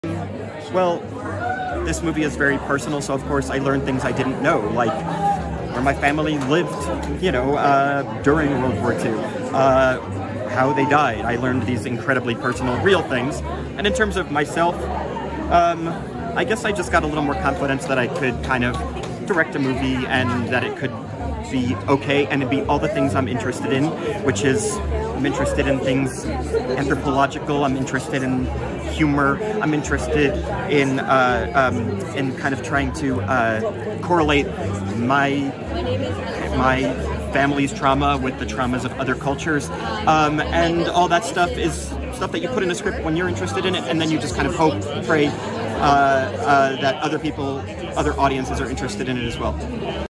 Jessie Eisenberg and A Real Pain at Sundance Film Festival
At the awards ceremony for the 2024 Sundance Film Festival, Jessie Eisenberg discusses what he learned about himself from working on his film A Real Pain.